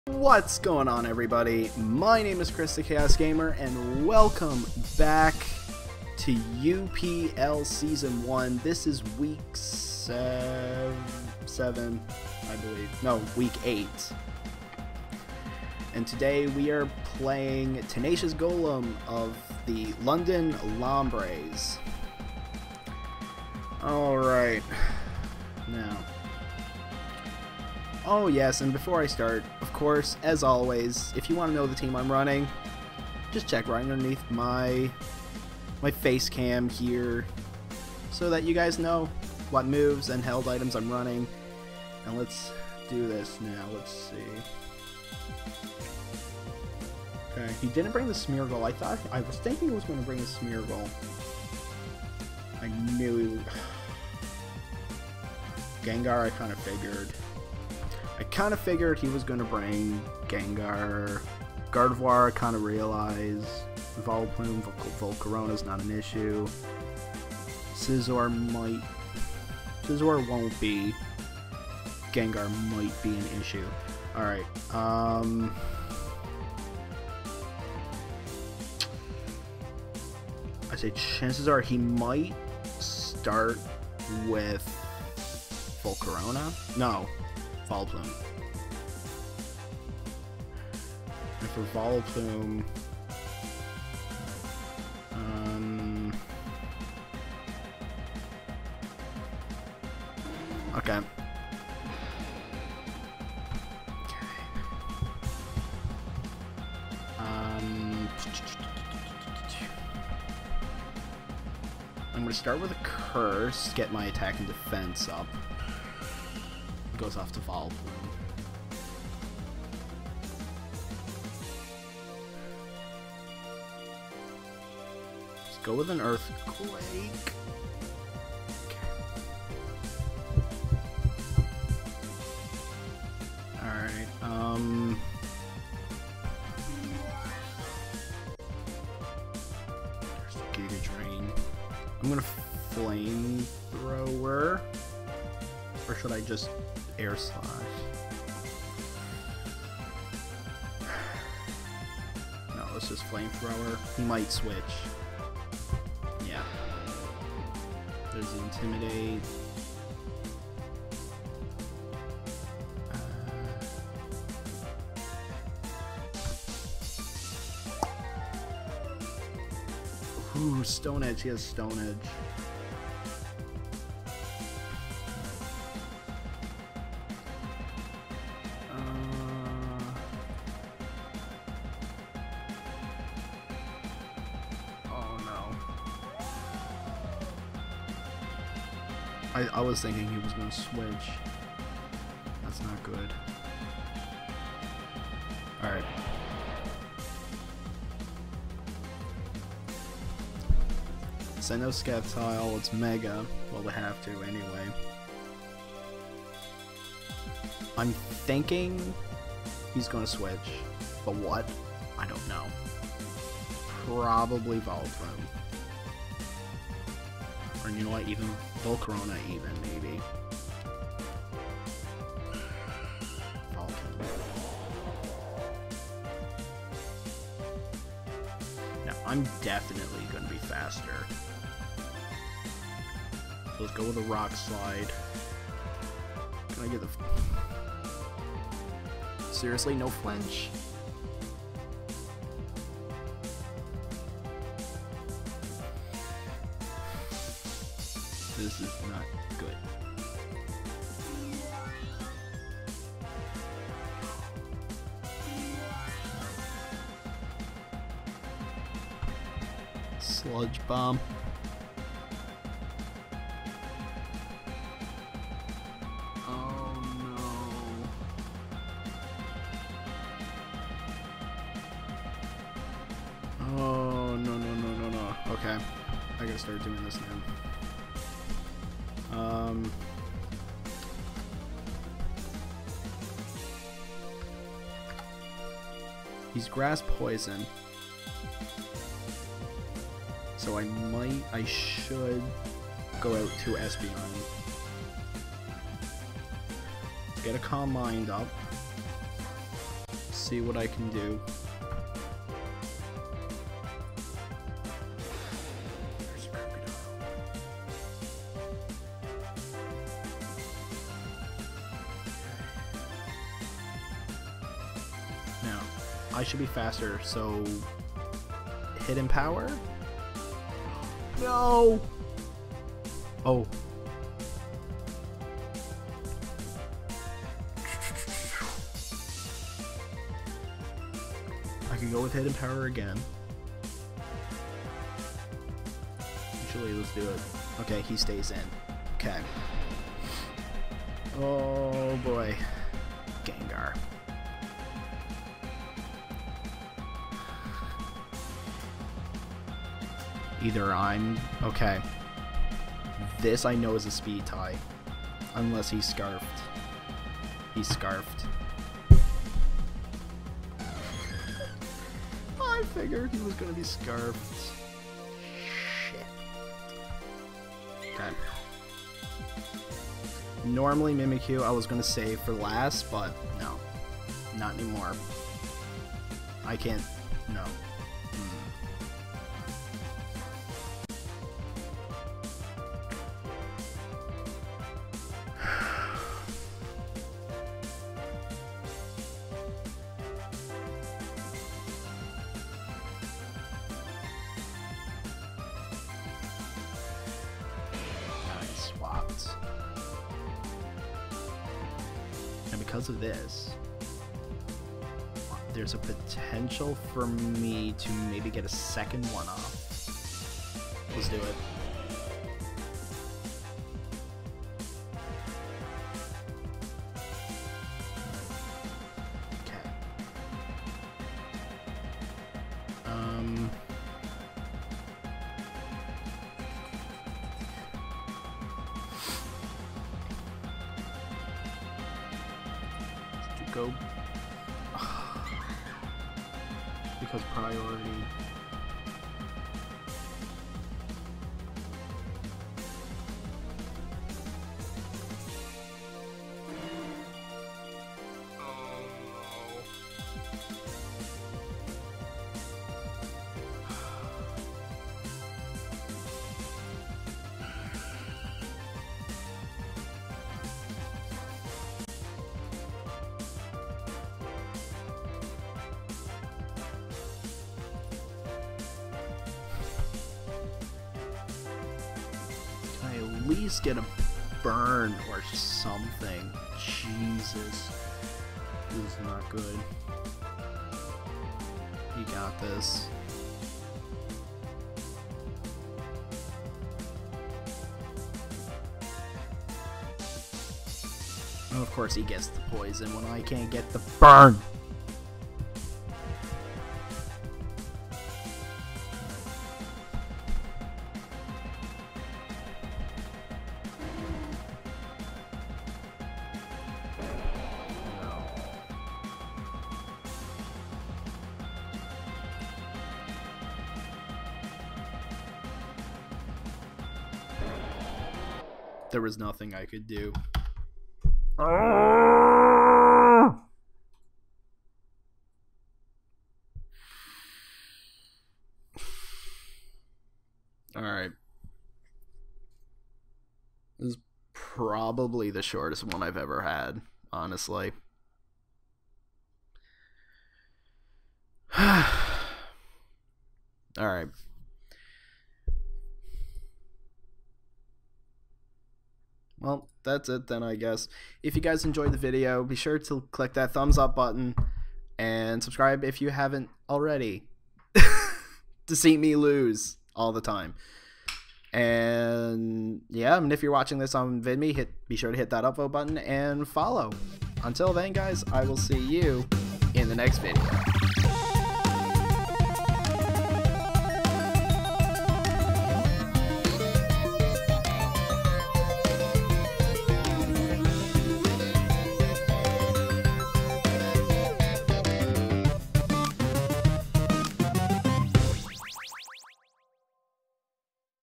UPL S1W8 - St. John's Sharpedos vs London Lombres
Still searching for that first win, we take on TenaciousGolem

🎥 Other Ways to Watch Me 🎥

🎬 Music and Credits 🎬